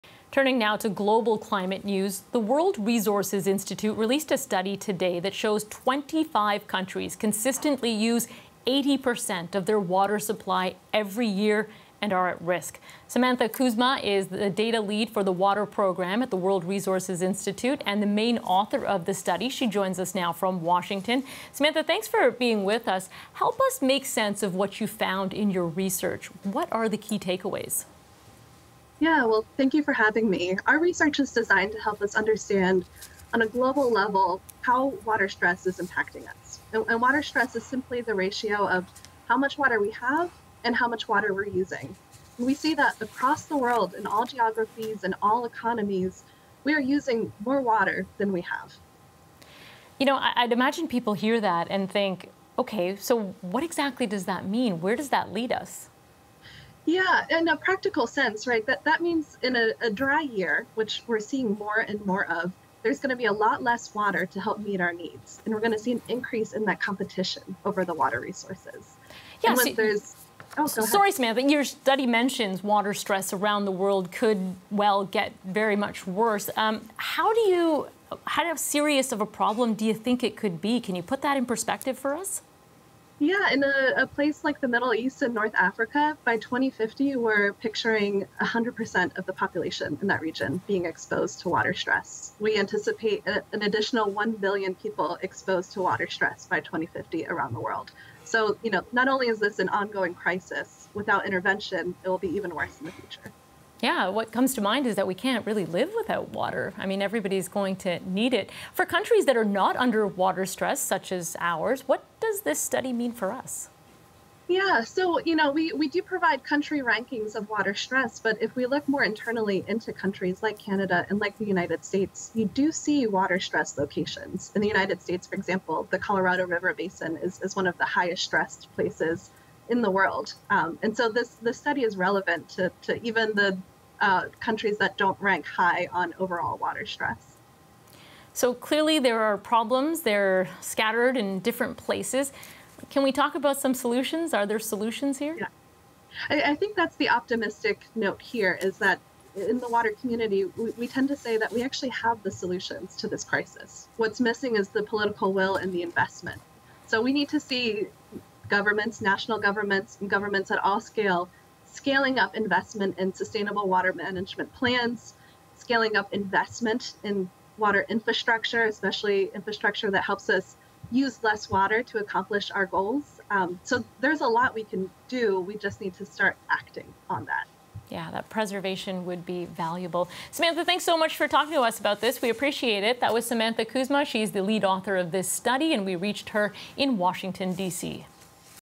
Dozens of countries using most of their water supply yearly, new study shows
The World Resources Institute released a new study that shows 25 countries are using 80 per cent of their water supply every year. Samantha Kuzma, the study's main author, says humans are using more water than ever, and water stress around the world could continue to worsen as a result.